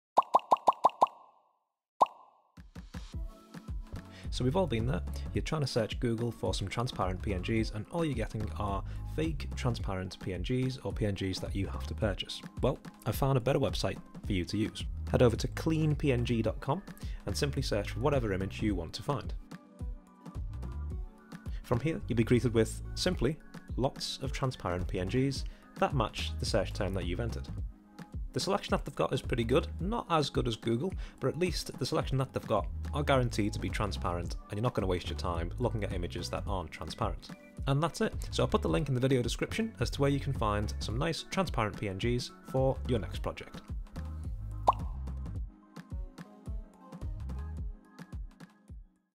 Where to Find Free Transparent PNG Images
Struggling to find free transparent PNGs for your project, take a look

Please note: This video isn't sponsored, this is just a useful website that I've used before.